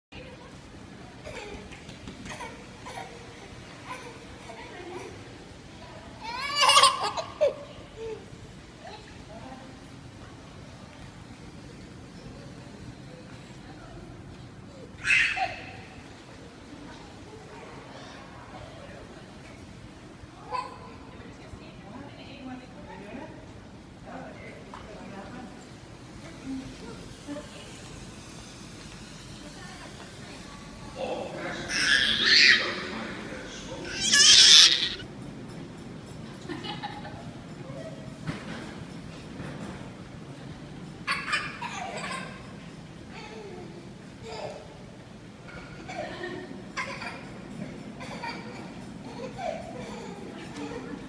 Your Adan Minute
Adan enjoys himself at Bradley Airport.